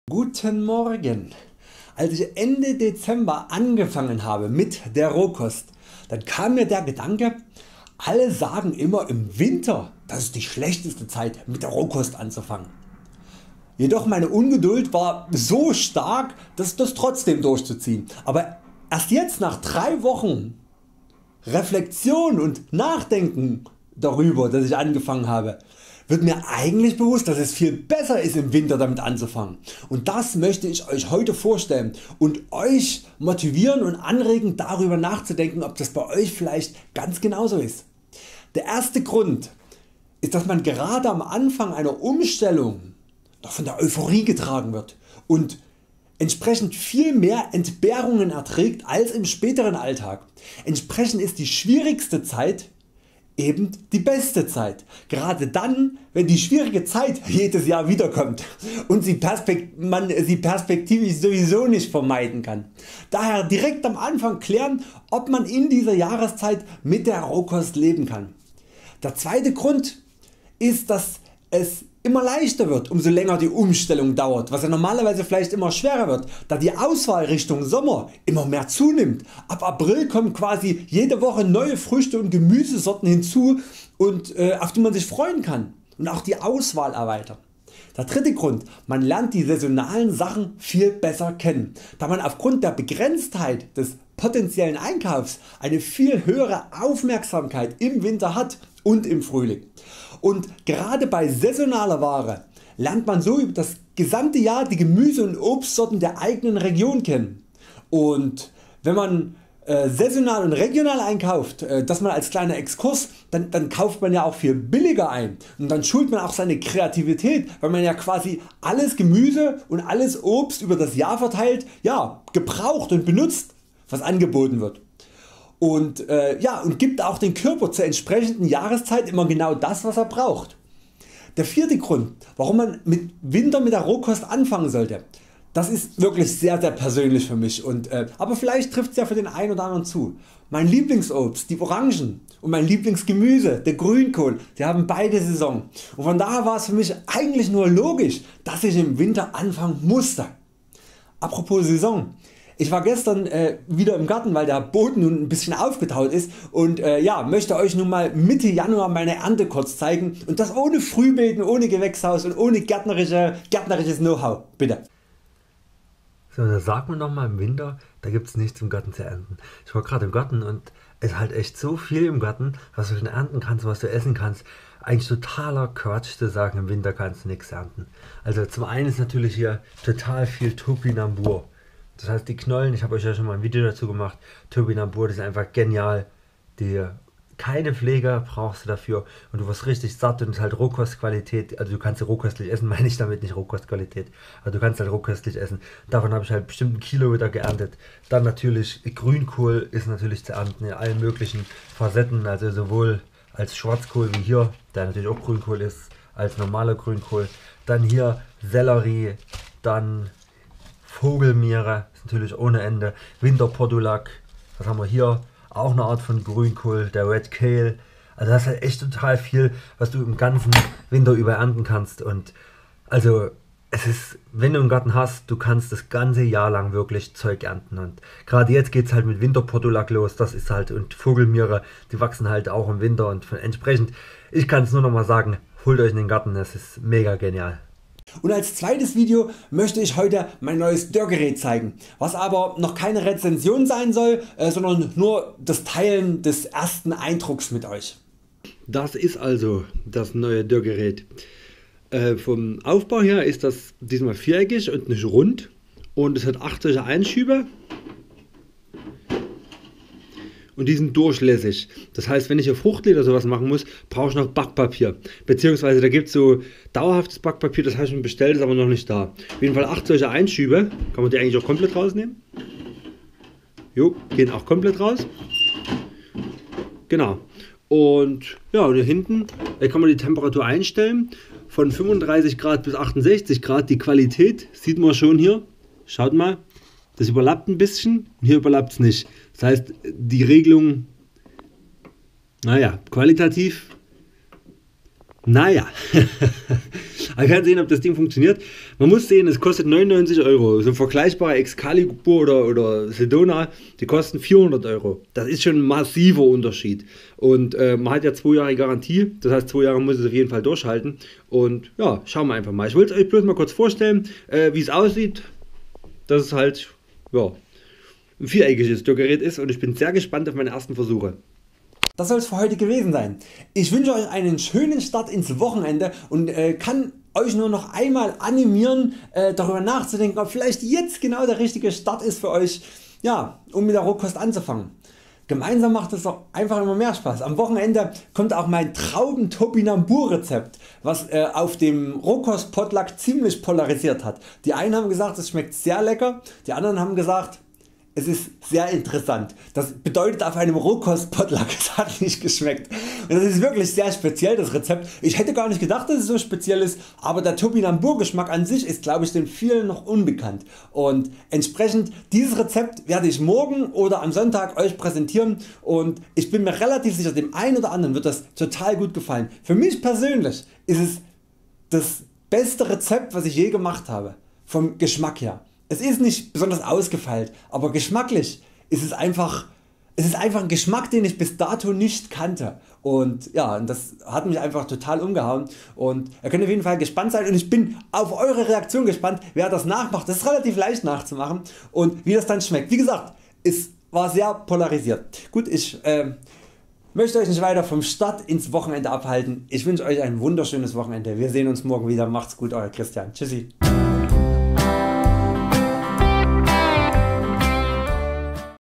4 Gründe im Winter mit Rohkost Ernährung anzufangen! & Neues Dörrgerät
Winter und gesunde Rohkost Ernährung passt nicht? Von wegen ;). Heute nenne ich 4 Gründe warum für mich der Start im Winter besser ist, als im Sommer! Dazu zeige ich Euch mein neues Dörrgerät ;)